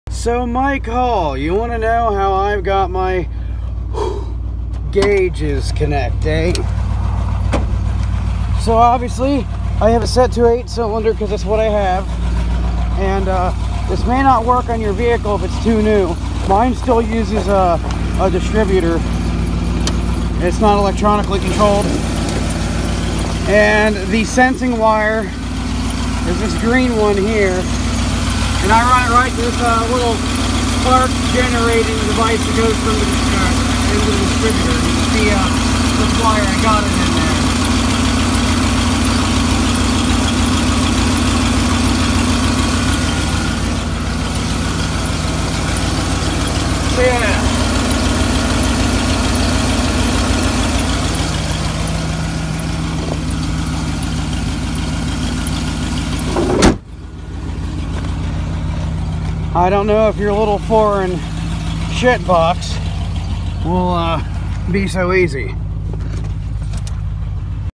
Were my after market tac gets signal on 1996 F-150 5.0 302
video was for request of person in discord server